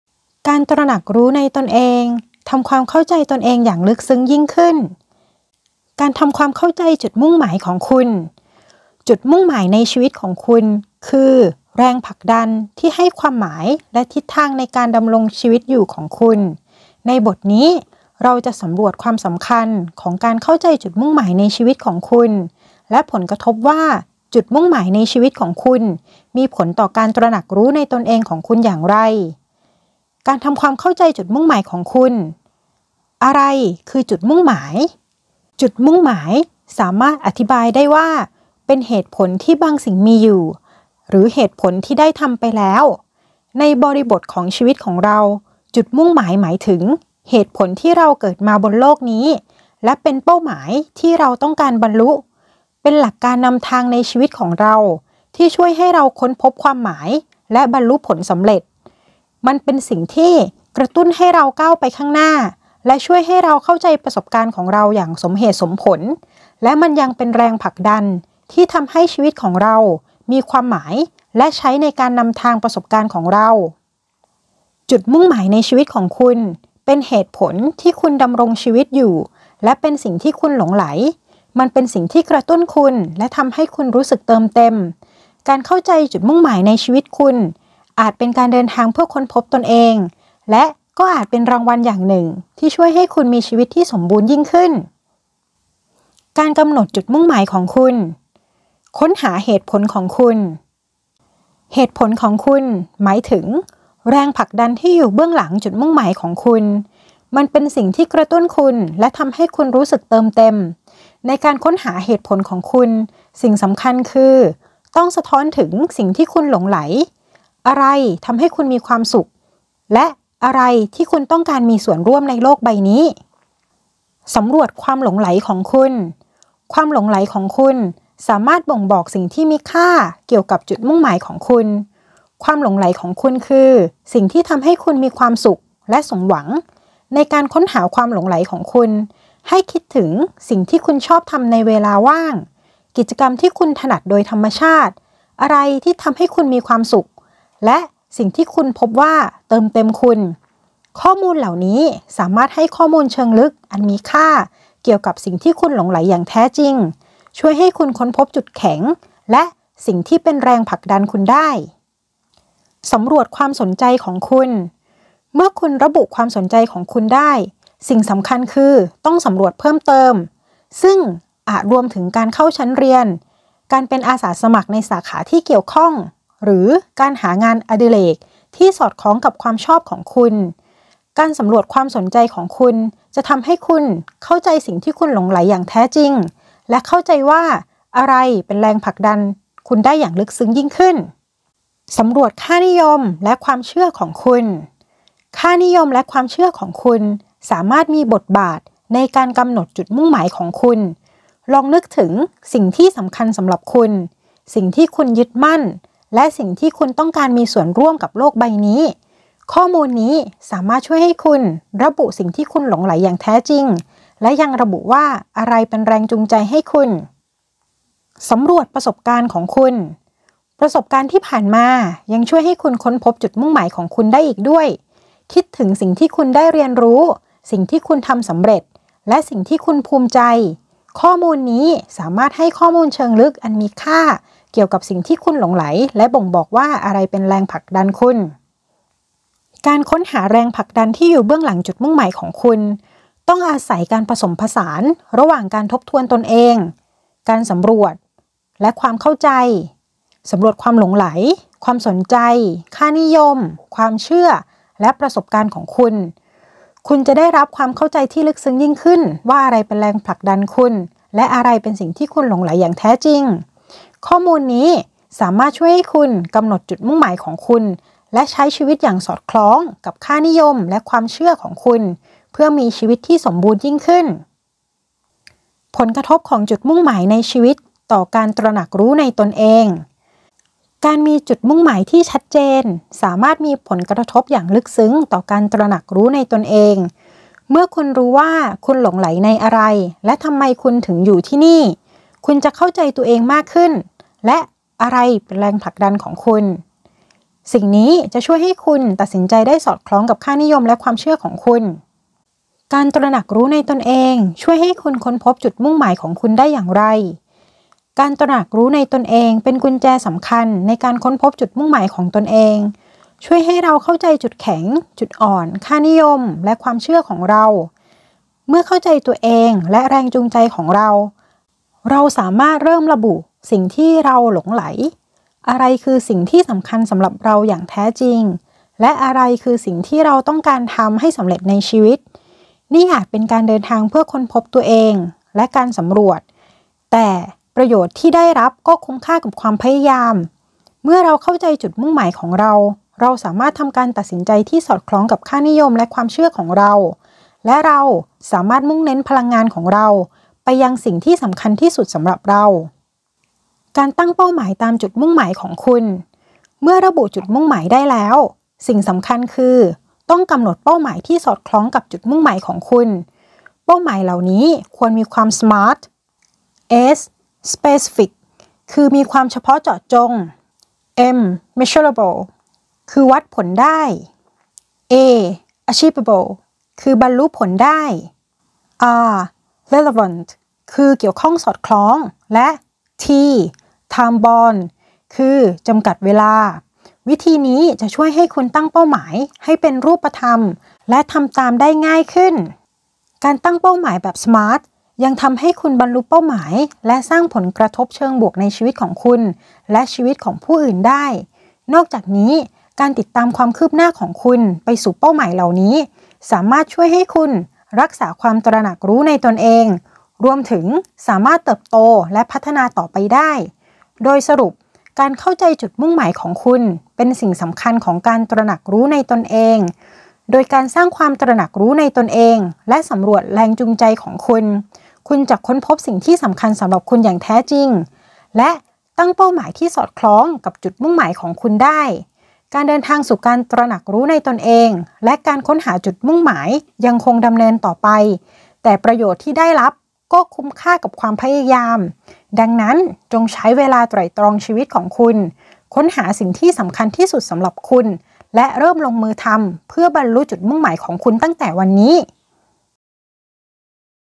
การตระหนักรู้ในตนเอง : เข้าใจจุดมุ่งหมายของคุณ
การตระหนักรู้ในตนเอง : ทำความเข้าใจตนเองอย่างลึกซึ้งยิ่งขึ้น
การทำความเข้าใจจุดมุ่งหมายของคุณ
สนับสนุนการศึกษาเรียนรู้พัฒนาตนเองด้วยการฟังหนังสือเสียงระหว่างเดินทาง หรือทำงานอดิเรกที่ไม่ใช้สมาธิมากนัก เป็นการใช้เวลาที่มีให้เป็นประโยชน์มากขึ้น
เพื่อเรียนรู้และเพิ่มศักยภาพของตนเองตลอดเวลาตลอดไปตลอดชีวิต